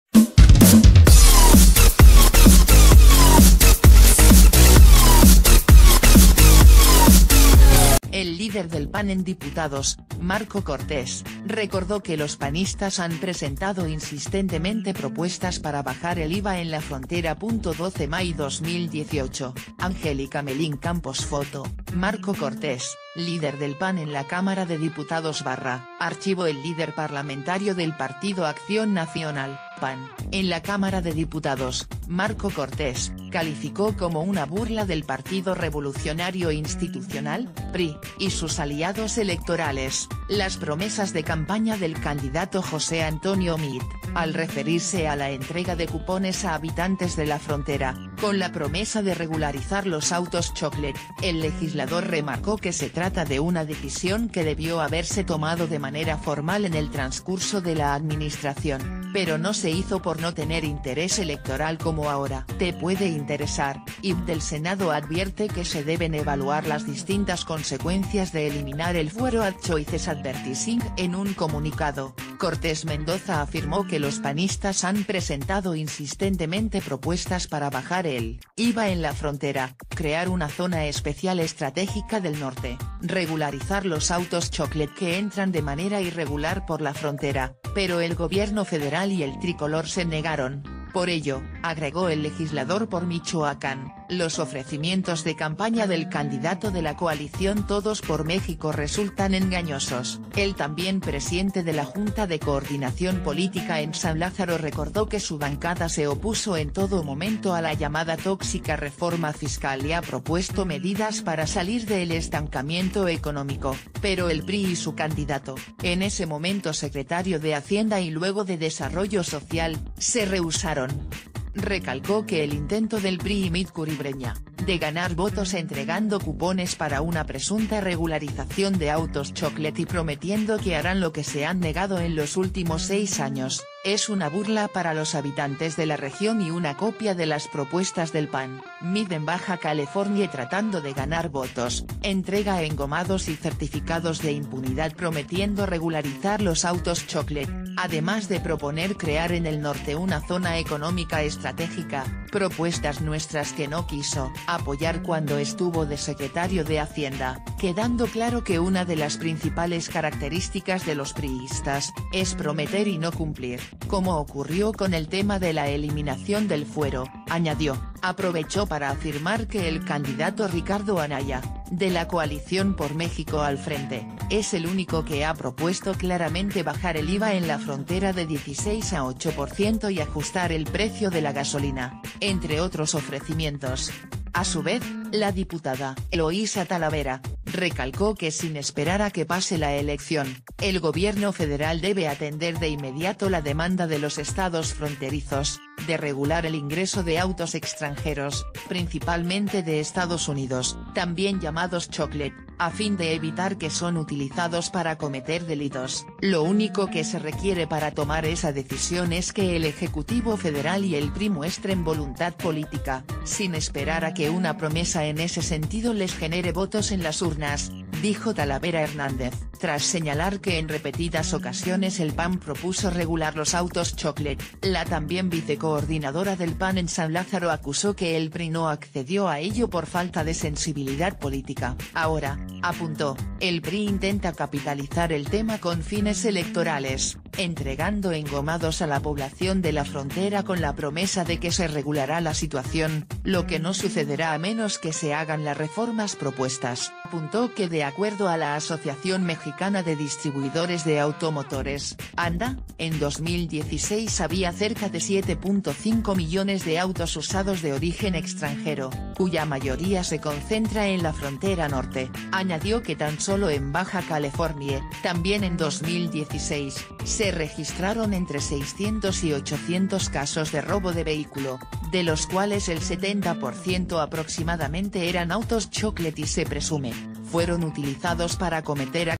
Las promesas de la campaña de Meade son "una broma", dice PAN en San Lázaro
El líder del PAN en Diputados, Marko Cortés, recordó que los panistas han presentado insistentemente...